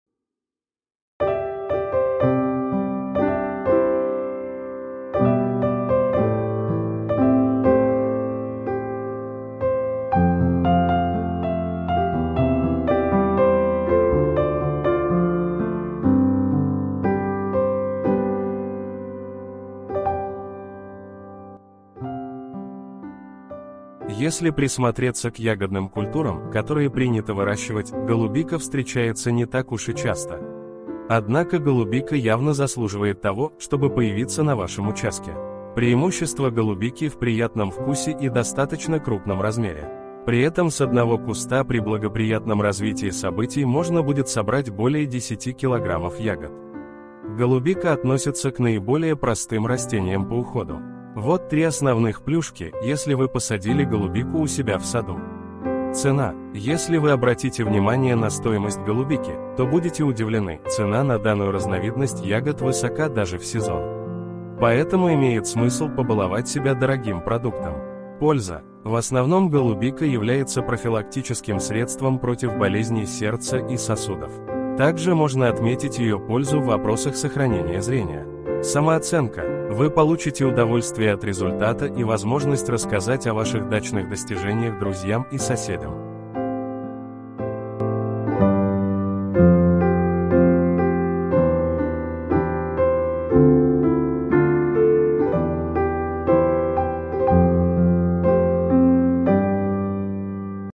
Video presentation
In this season, @HiTrends is expanding his wisdom and enhancing my abilities in the Tech Specialist teachings. This day, I transformed my demonstration from Power Point into a moving picture presentation. It speaks of three advantages of cultivating blueberries in your abode garden. If it pleases you, I am in full favor of it.